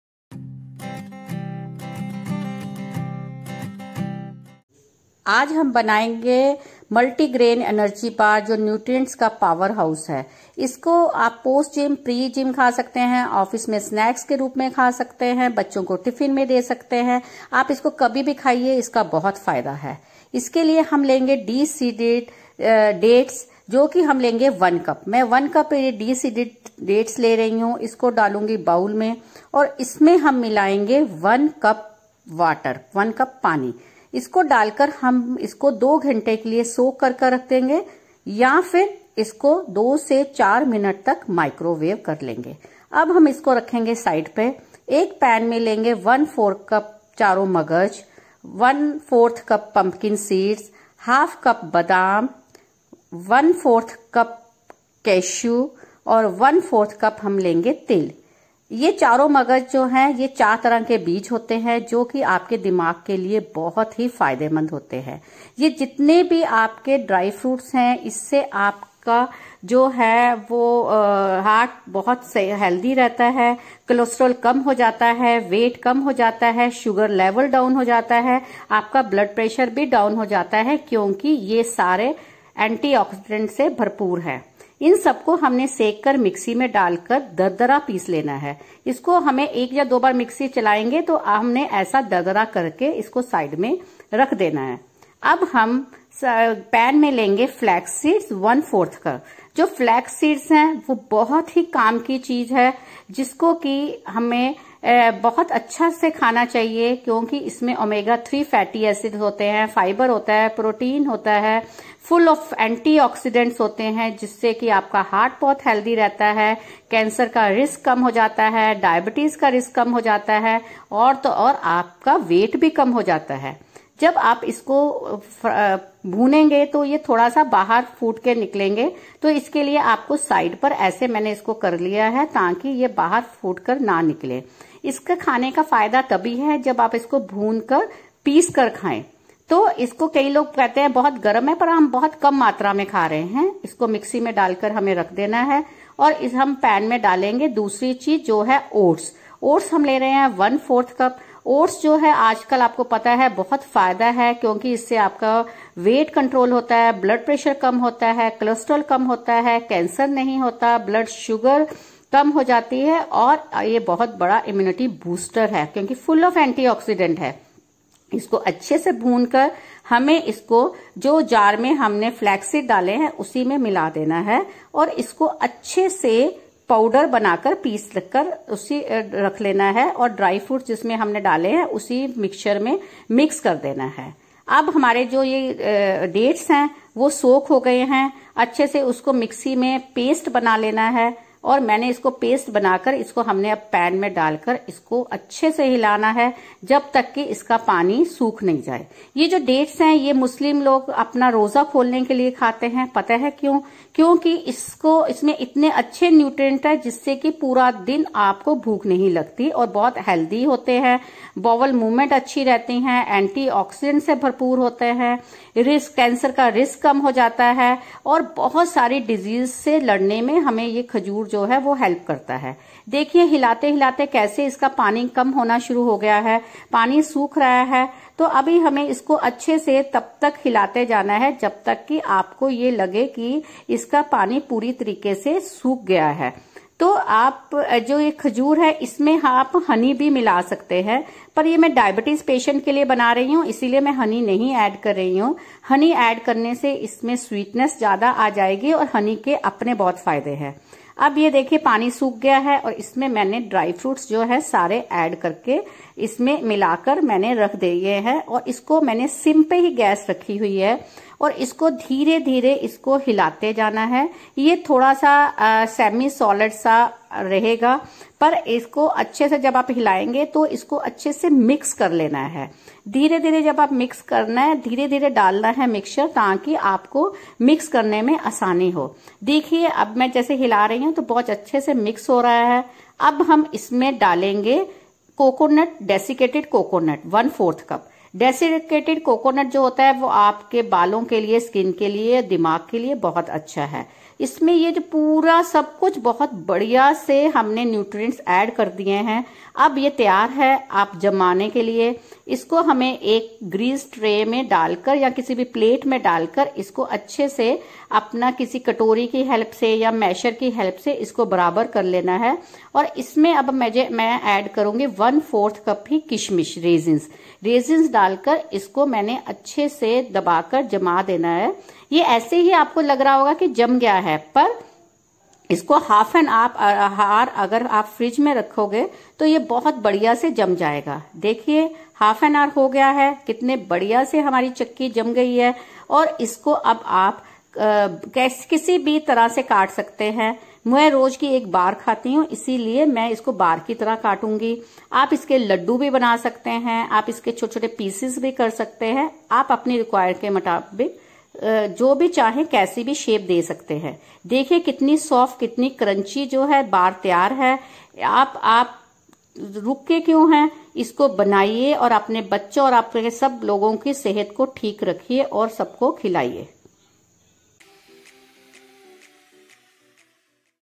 Energy Bar | No Bake Granola Bar | Sugar-free Oil Free | Healthy Bar
Don’t we all love to indulge in guilt free eating?
Presenting to you sweet snack bars loaded with nutrition and health benefits. You don’t have to think before gulping them down!
Yes, you read that right.

Your body shall love you for feeding it with these energy power houses - be it before or after gym workouts, as snacks or for fulfilling the midnight cravings.
This is a gluten-free cereal bar filled with oats, nuts and seeds, making it not only rich in proteins,  fiber and omega 3 fatty acids but also, a tasty treat for all ages.

Sweetened with dates, made with pure love - 100% natural ingredients and 0% preservatives!

Do try them and leave your feedback in the comments below!

Ingredients

Deseeded Dates 1 Cup
Char Magaz 1/4 Cup
Almond 1/2 Cup
Pumpkin seeds 1/4 Cup
Cashew 1/4 Cup
Sesame seeds ( Til ) 1/4 Cup
Flax seeds 1/4 Cup
Desicated coconut 1/4 Cup